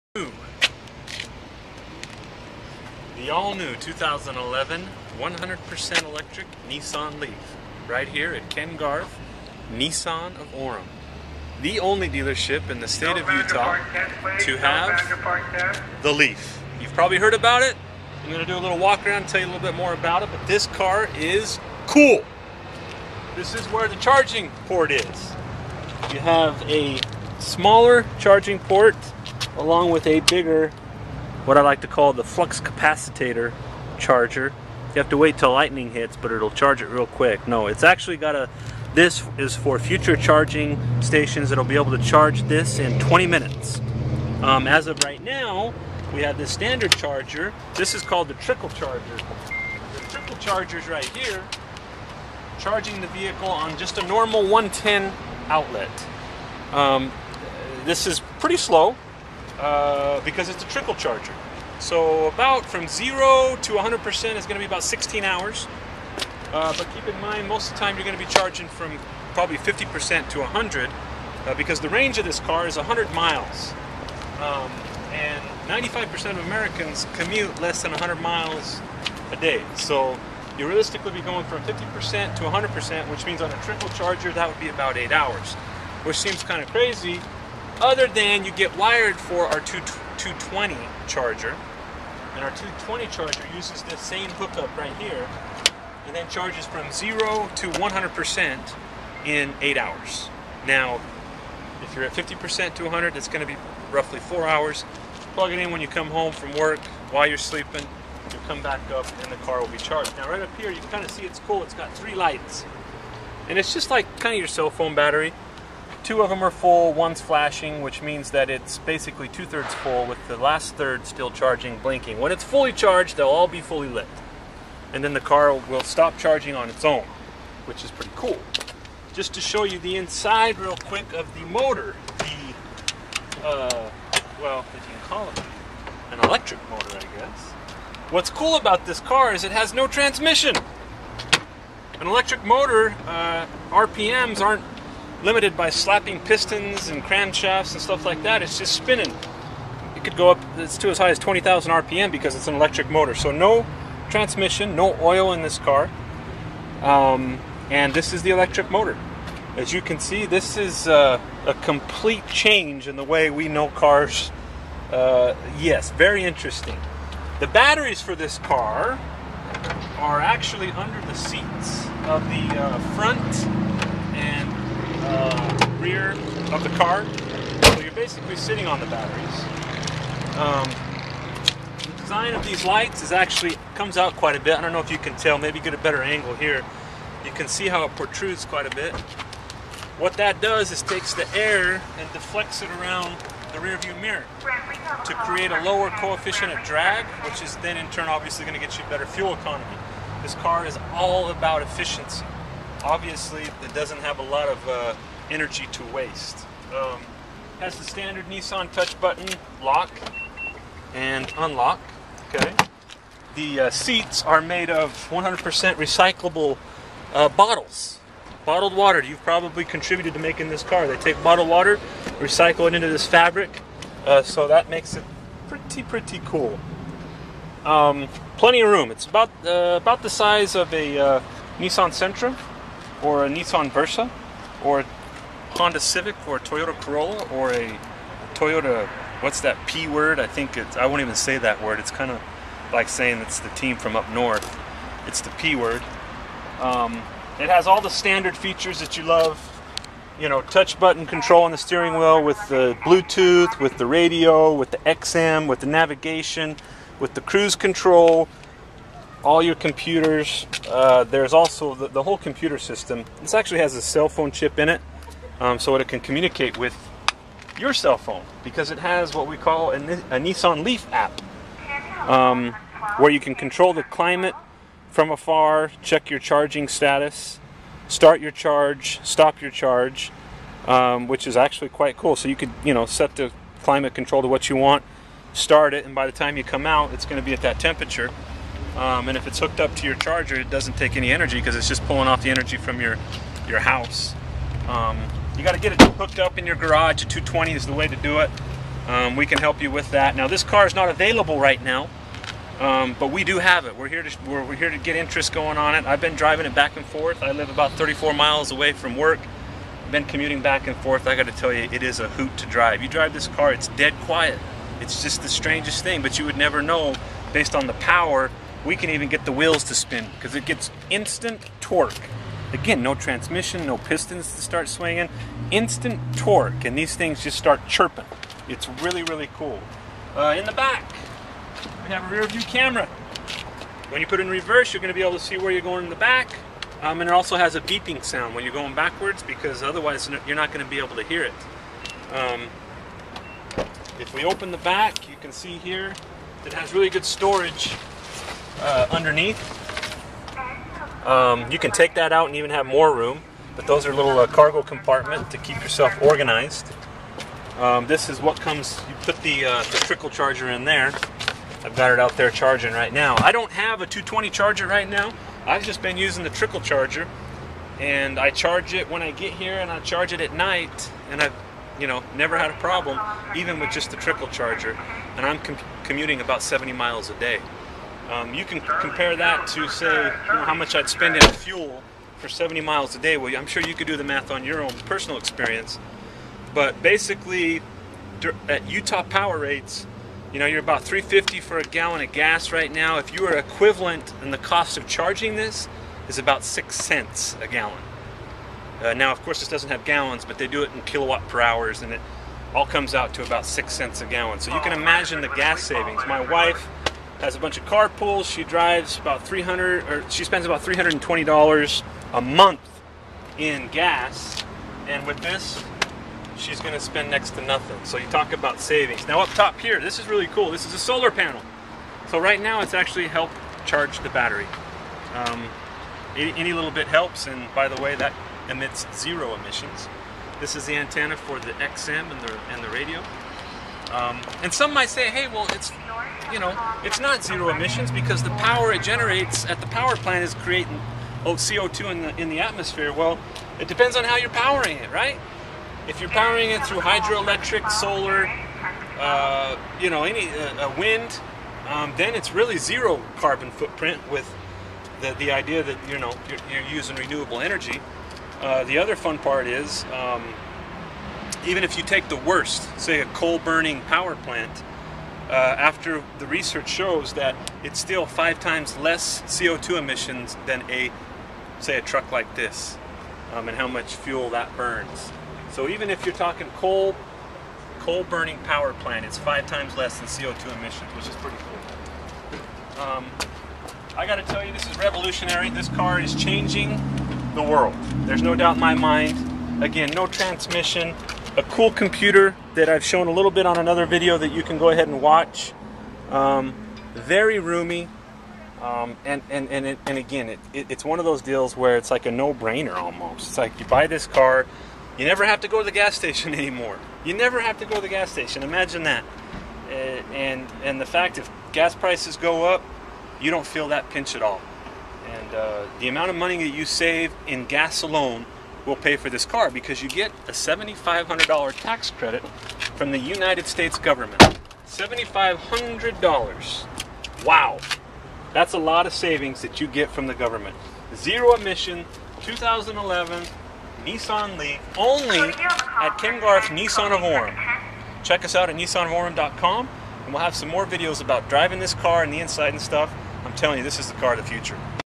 Nissan Leaf walkaround - Ken Garff Nissan of Orem Utah 84058
Tyler Slade, General Manager at Ken Garff Nissan of Orem, gives a great walk around on the all electric, zero-emissions Nissan Leaf. We are the first Nissan dealer in the state of Utah to have one on our lot. (Sorry, not available for sale.) Another video with details of the interior coming soon!